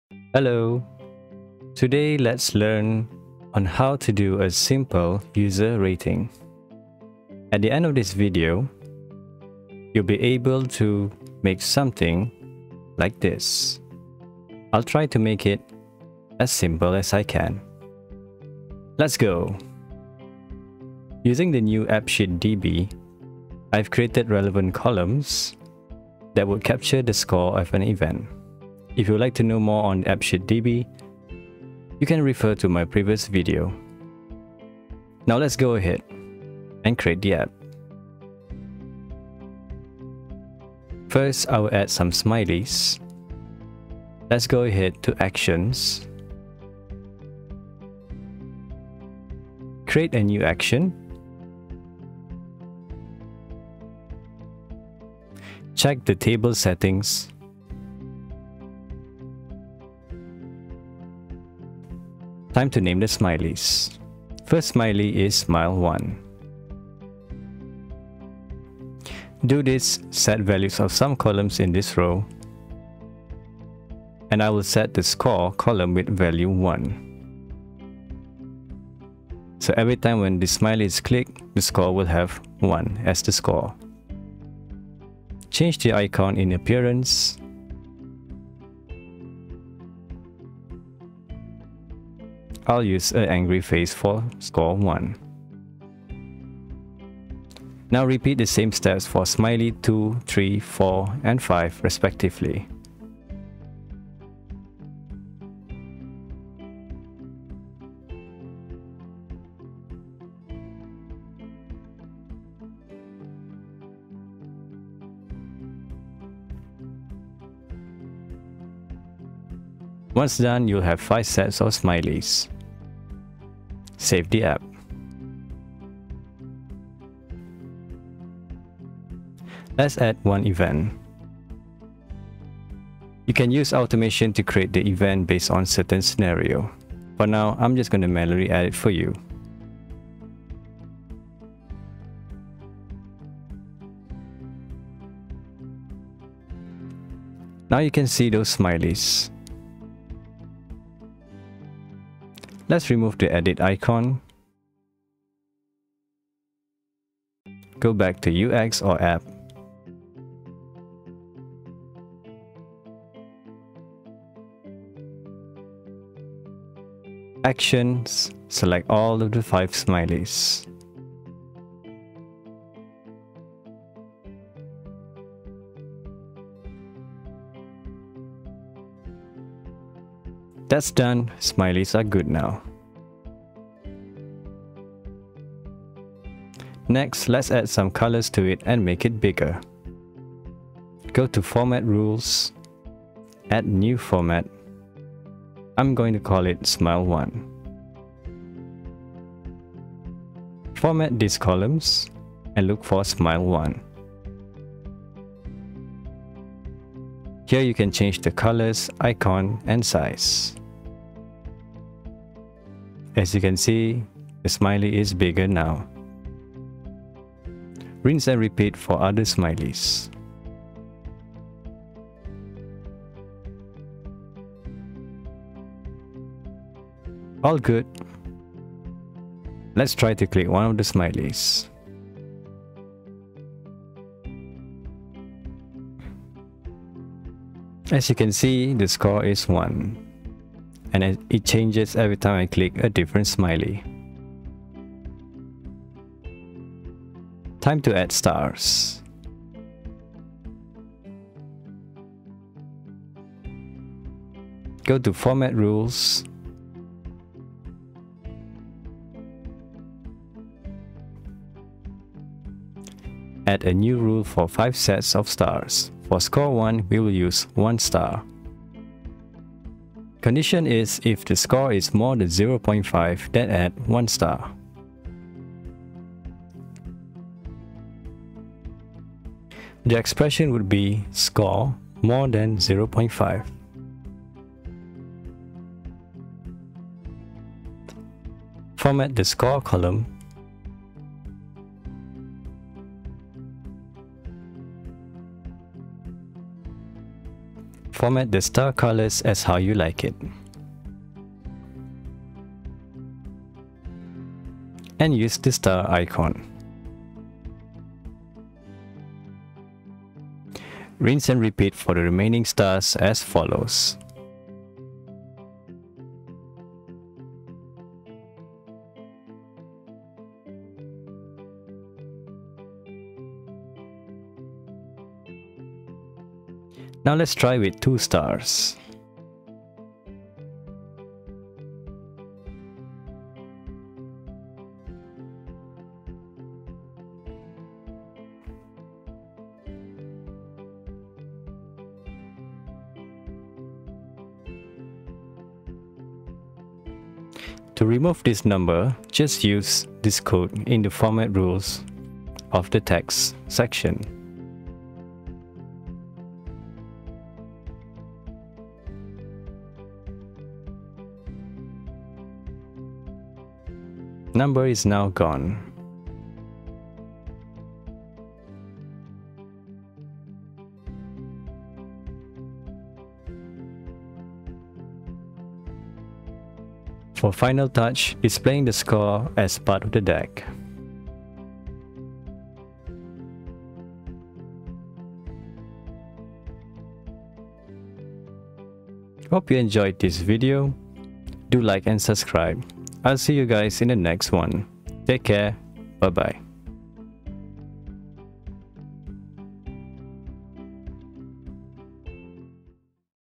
Appsheet Episode 22: How to make custom User Rating. Measure your performance today!
User rating is an important part of a service. This is to measure the user experience of your clients or customers. Here's a basic tutorial on how to make a user rating through Appsheet. At the end of this video, you will be able to create your very own user rating module.